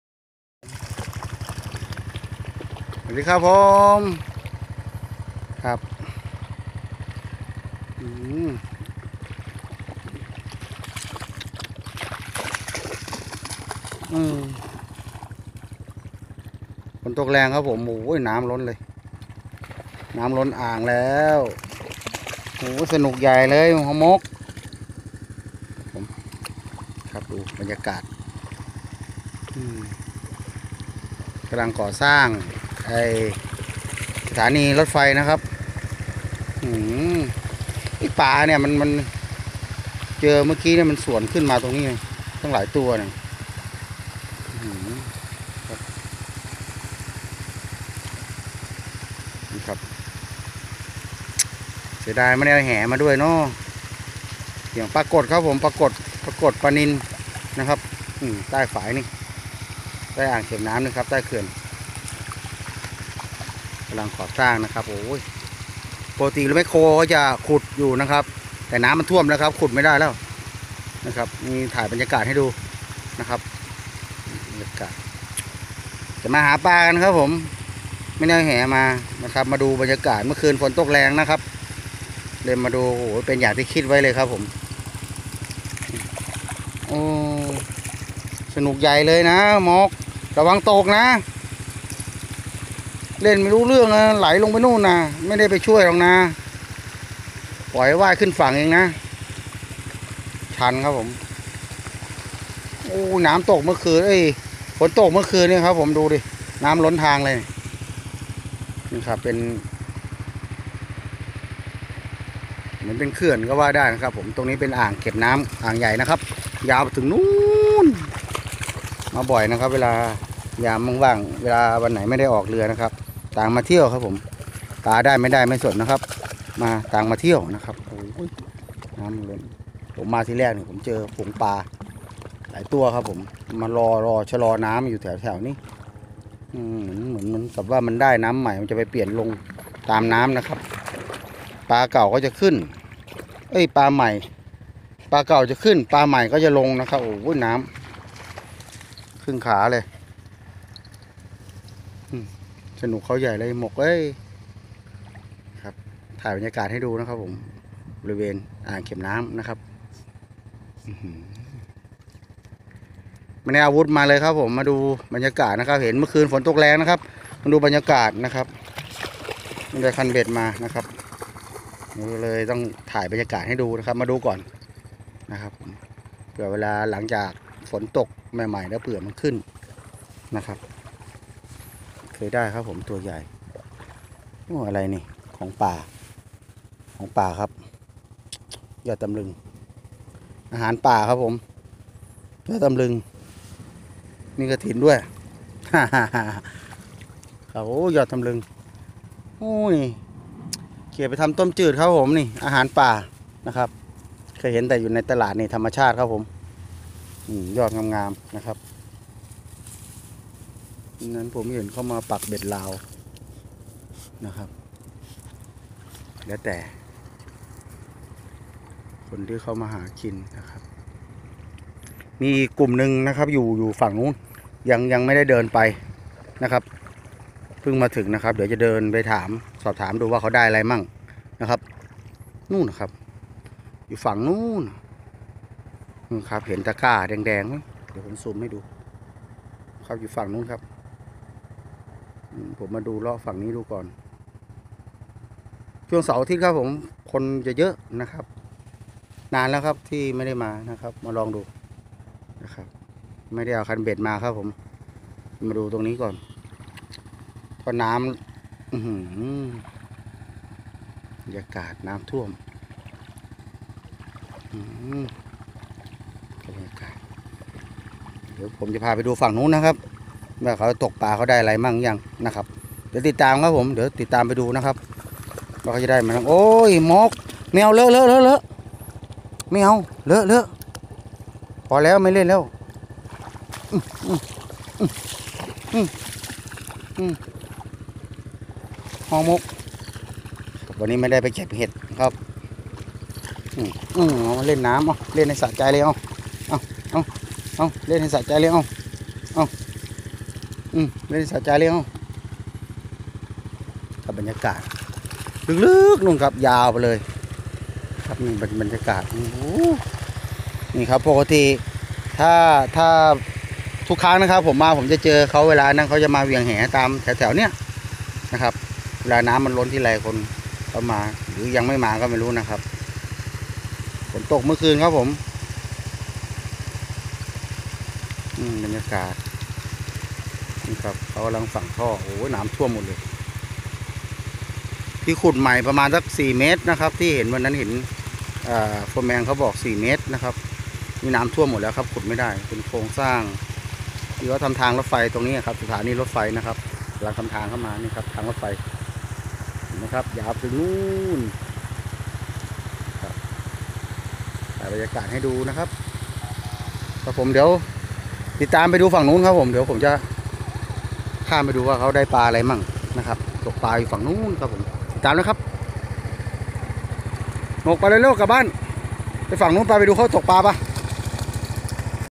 ฝนตกคืนเดียว น้ำล้นอ่าง
บรรยากาศอ่างเก็บน้ำ ฝนตกมาคืนเดียวน้ำล้นอ่าง